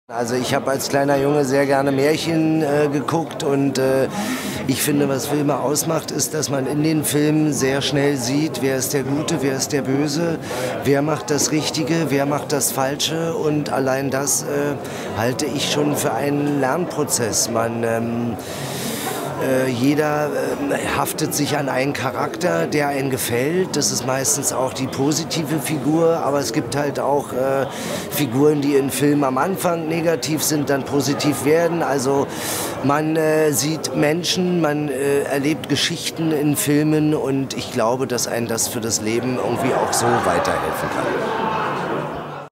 Oliver Korittke
Filme können bei der Persönlichkeitsentwicklung eine große Rolle spielen, weiß Schauspieler Oliver Korittke ("Doktorspiele", "Wilsberg") FILM+SCHULE NRW traf ihn am Set von "Wilsberg".